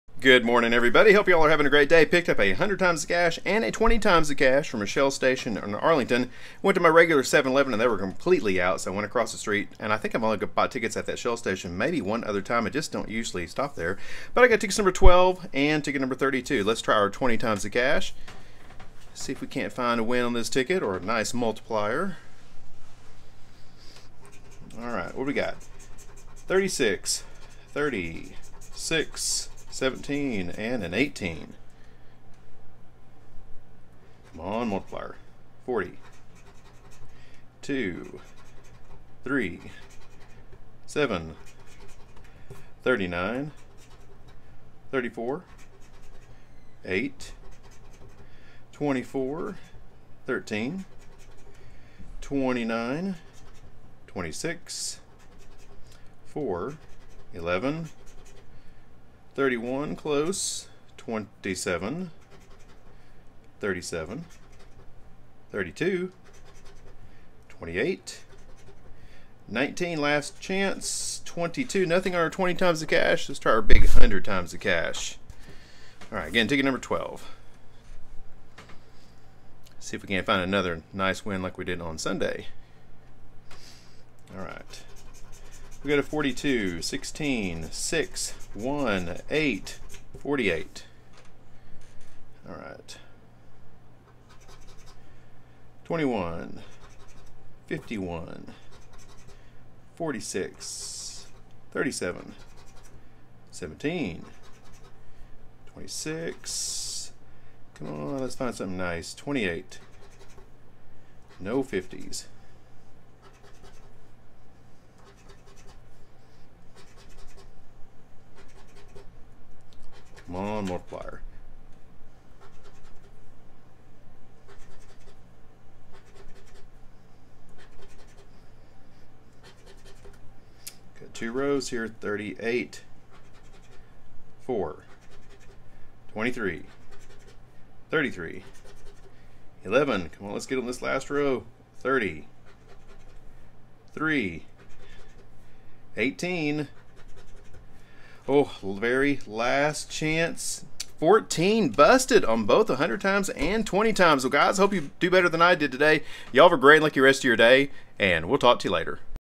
$20 100X The Cash! + 20X The Cash! 💰 TEXAS LOTTERY SCRATCH OFF TICKETS
PLAYING THE SCRATCH OFF GAMES OF TEXAS!

WANT TO SEND ME MAIL? SEND IT HERE:
FIXIN TO SCRATCH